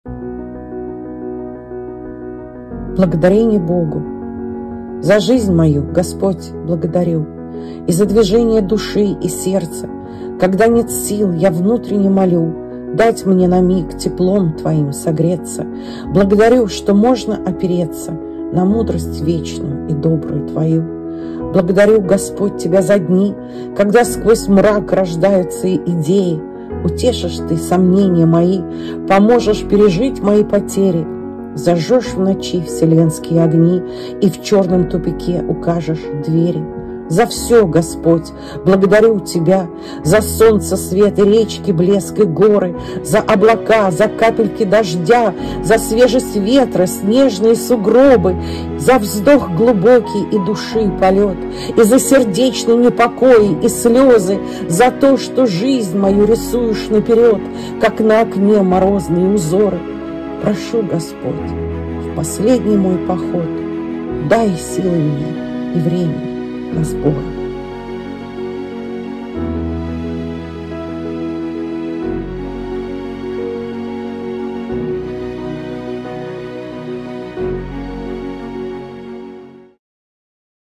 БЛАГОДАРЕНИЕ БОГУ! Мои новые стихи и фотографии из моего архива.
Эти стихи моя благодарность Господу и эти фотографии сделаны мною в разных местах вокруг нашего дома и деревни, где мы живем- каждый день я вижу красоту природы и Благодарю Господа за возможность лечить душу глядя на его прекрасный мир!

БЛАГОДАРЕНИЕ БОГУ!

За жизнь мою, Господь, благодарю,
И за движение души и сердца,
Когда нет сил - я внутренне молю,
Дать мне на миг теплом Твоим согреться,
Благодарю, что можно опереться
На Мудрость Вечную и Добрую Твою.

Благодарю, Господь тебя за дни,
Когда сквозь мрак рождаются идеи,
Утешишь Ты сомнения мои,
Поможешь пережить мои потери,
Зажжешь в ночи Вселенские огни,
И в чёрном тупике укажешь двери.

За все, Господь, благодарю Тебя!
За солнца свет и речки блеск и горы,
За облака, за капельки дождя,
За свежесть ветра, снежные сугробы,
За вздох глубокий и души полёт,
И за сердечный непокой и слёзы,
За то, что жизнь мою рисуешь наперёд,
Как на окне морозные узоры...

Прошу, Господь, в последний мой поход:
Дай силы мне и времени на "сборы".

Дождливое утро 22 декабря 2022 год.